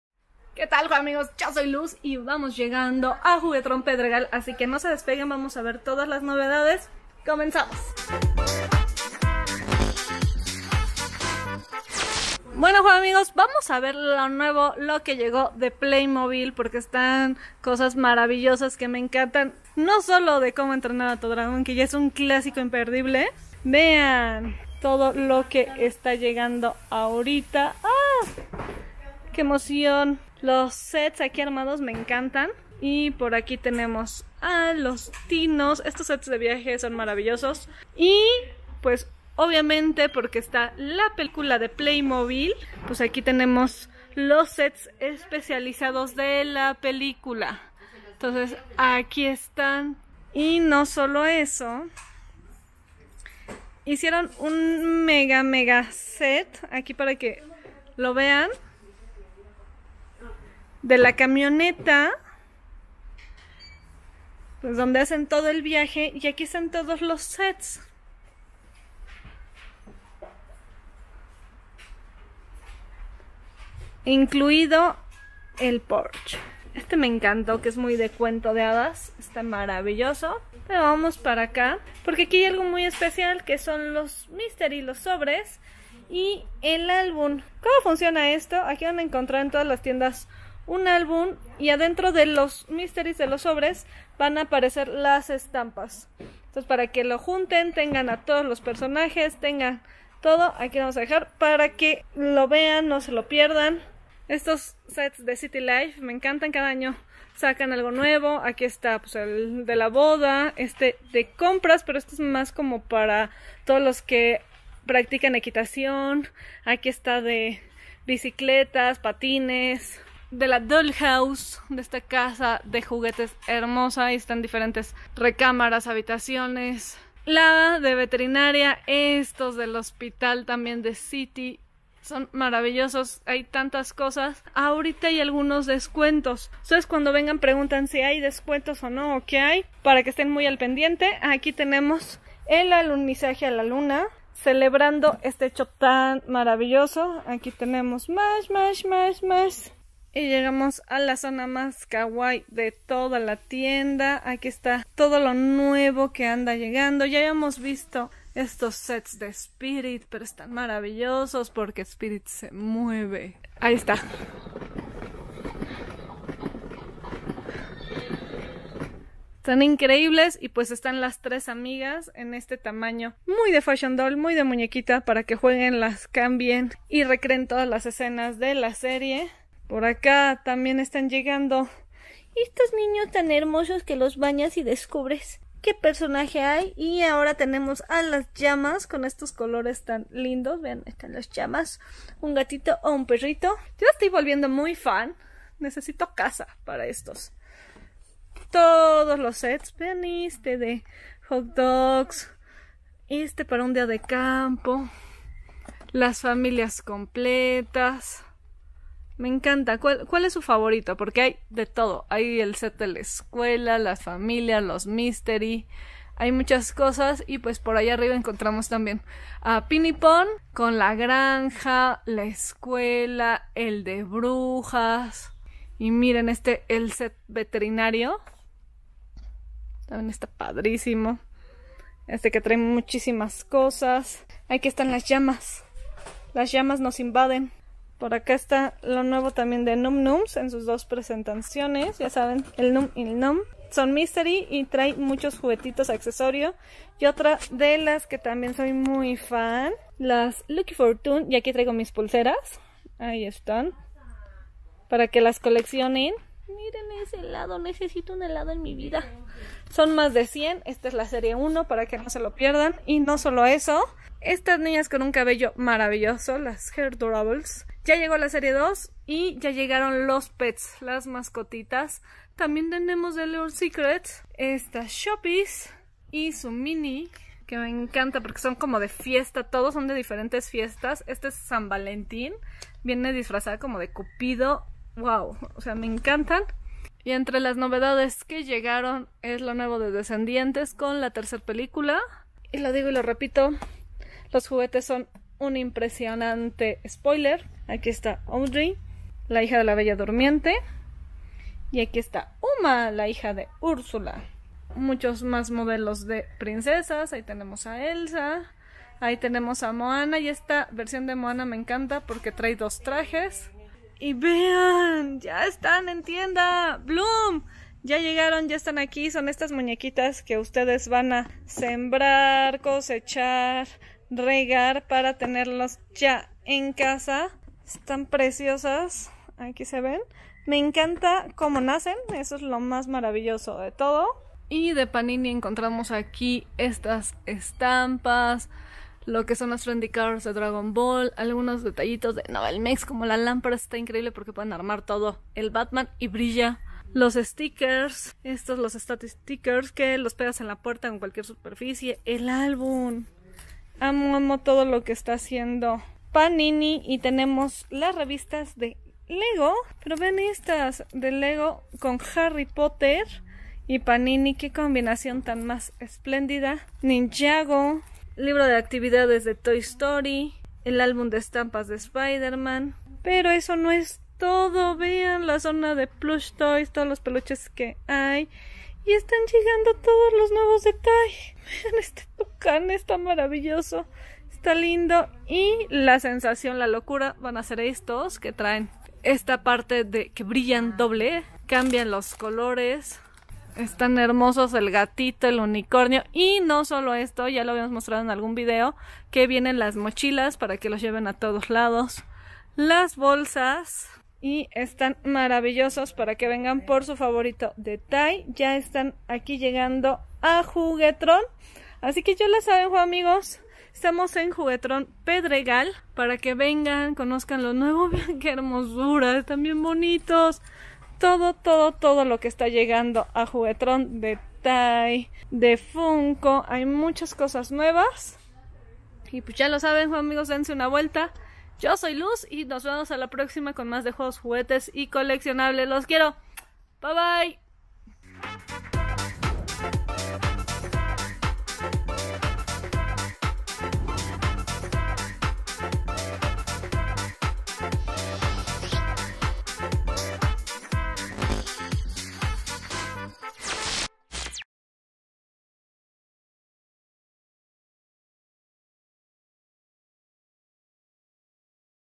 Juguetron Pasillo de Niñas ► Juegos Juguetes y Coleccionables
lo nuevo de playmobil la pelicula y las novedades de juguetes para niñas en Juguetron Pedregal